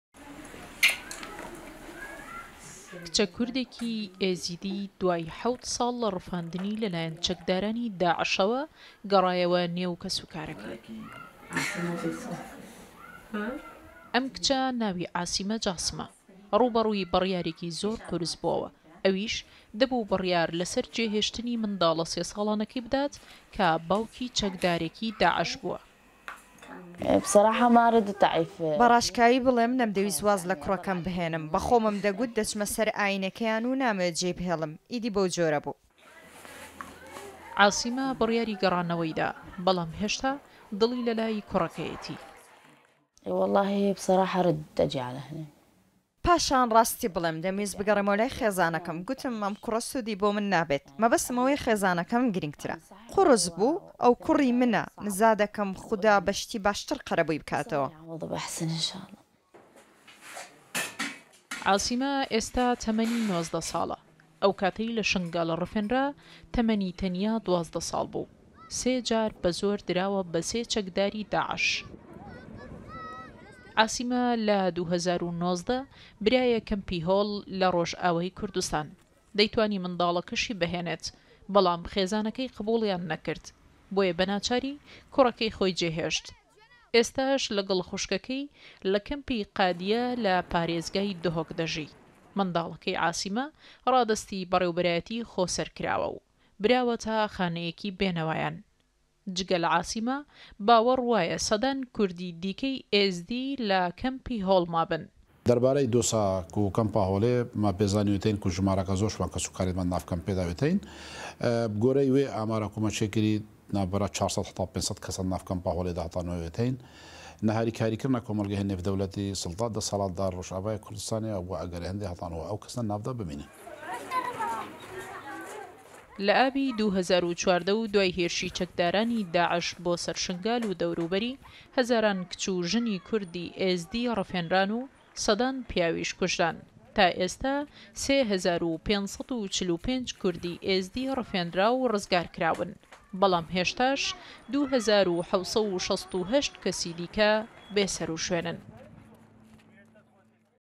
منداڵەکەی جێدەهێڵێت و دەگەڕێتەوە ناو کەسوکاری
کچە کوردێکی ئێزدی بەناوی عاسیمە جاسم دوای حەوت ساڵ لە رفاندنی لەلایەن چەکدارانی داعشەوە، منداڵە سێ ساڵانەکەی جێهێشت و گەڕایەوە نێو کەسوکارەکەی.